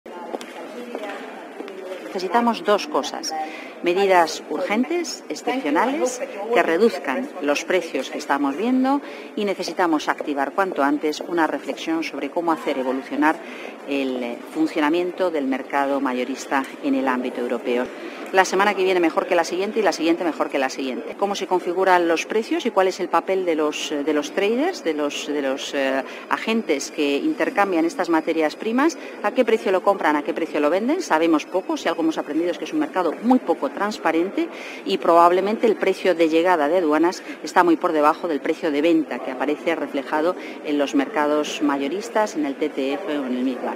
Teresa Ribera en el Consejo de Ministros de energía en Bruselas 🇪🇺
Una de las prioridades de la VP Teresa Ribera es la reforma del mercado mayorista de la energía para reducir precios y acabar con la falta de transparencia.